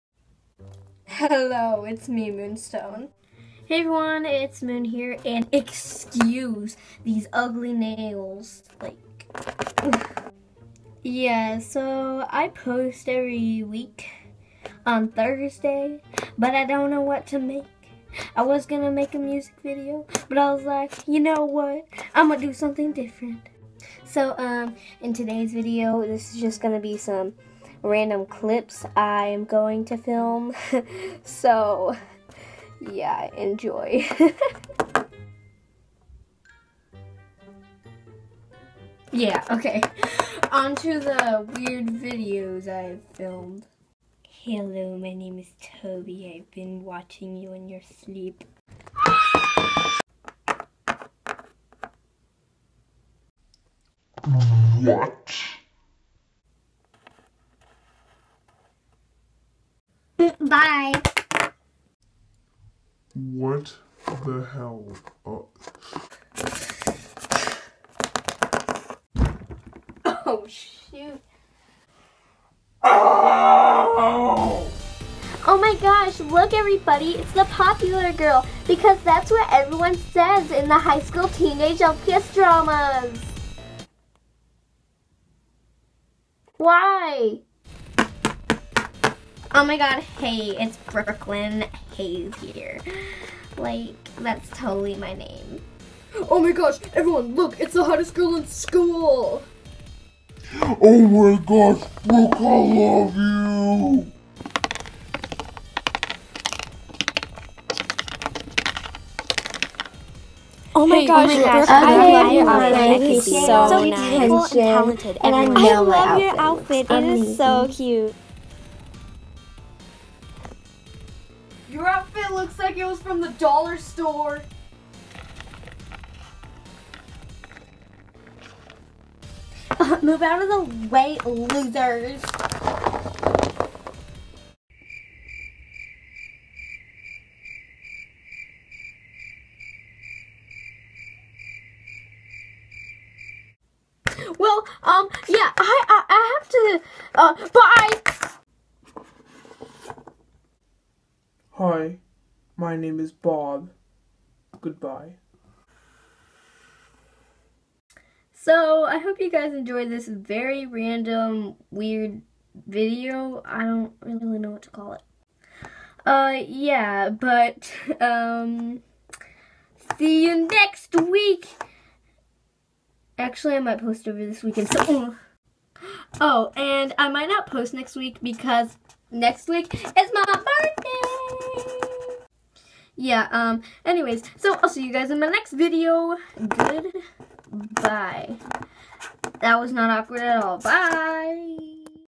I don't know what this is...
So...um. I didn't know what to post today so HOPE YOU ENJOYED THIS weird awkward thing I made😂😂😂
Actually half way through the video when I yelled my neighbors started looking at me like I was crazy...😂 ((lol I am))~Moon✨